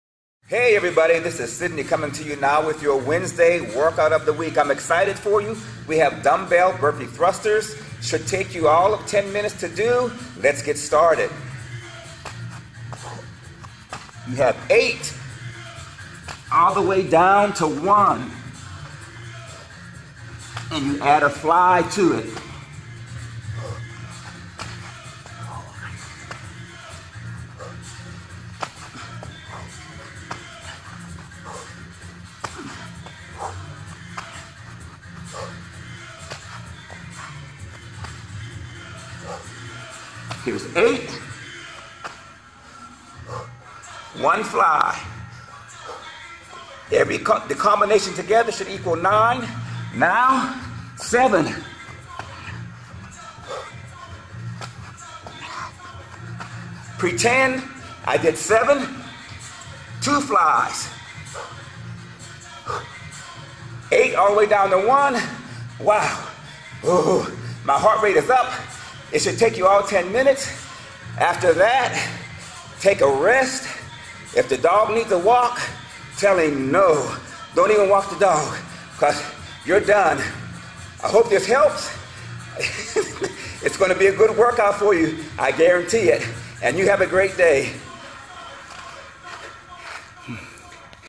WEDNESDAY WERKOUT OF THE WEEK 12-NOV-2014
WEDNESDAY WERKOUT OF THE WEEK

DB BURPEE THRUSTERS with FLY

Equipment needed: dumbbells

Time: 10 minutes. (you can also try adding this to an existing routine)

Little to no rest. Rest when you’re done!

Perform 8 reps of the DB Thruster, then do 1 fly
Perform 7 reps of the DB thruster, then do 2 flyes, etc
Repeat this exercise, decreasing reps on the DB thruster, and while increasing reps on the flyes. You’re finished when you reach 1 DB thruster and when you’ve done 8 flyes.

If doing 8 reps is too easy to start off with, start off with 12 reps and work your way down.

Modify as needed.

*note: oop! I want to point out that there is a mistake in this one , but I still think you’ll get a great werkout in! (I was too tired to re-shoot it again! LOL)

Good luck!